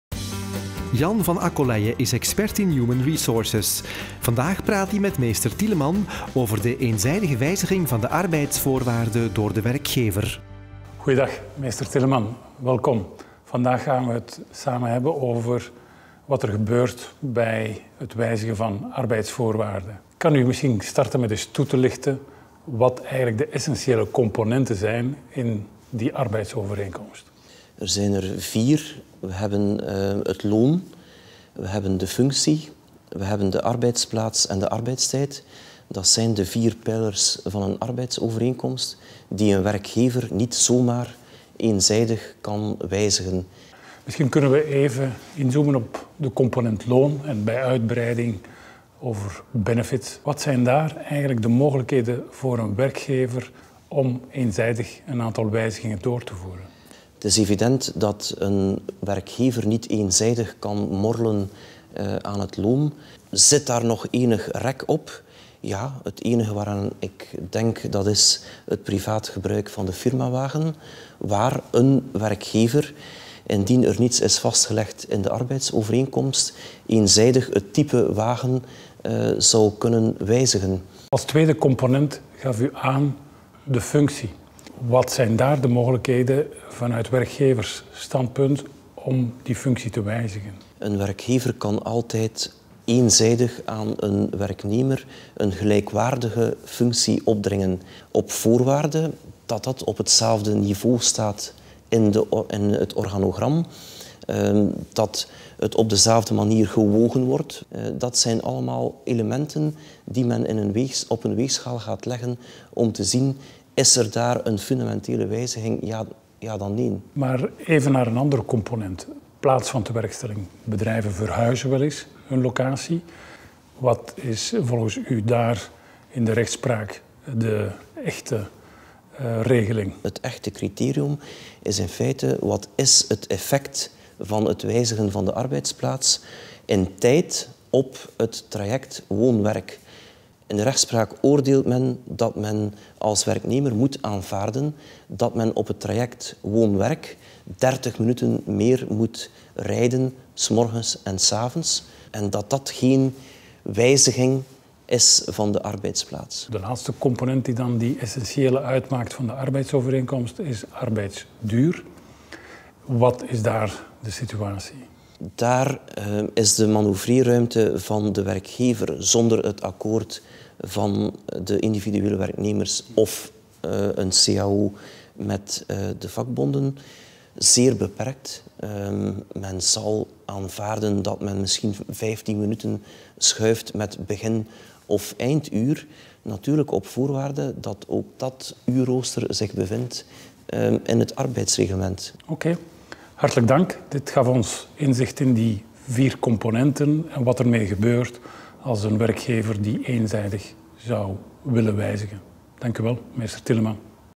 Arbeidsovereenkomst - Wijzigen arbeidsvoorwaarden door werkgever
De vier pijlers van de arbeidsovereenkomst zijn het loon, de functie, de arbeidsplaats en de arbeidstijd. Een werkgever kan die niet zomaar eenzijdig wijzigen.